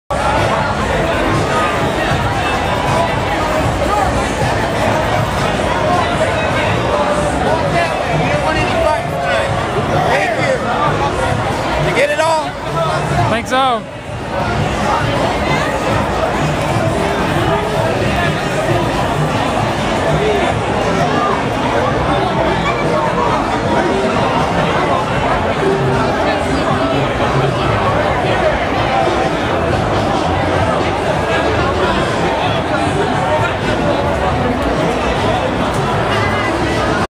Rude Austin Police Officer on 6th, 12/15/12
Videography by Lynn, of the Peaceful Streets Project.

I like that snarky "Did you get it all?" when he realizes he's being filmed.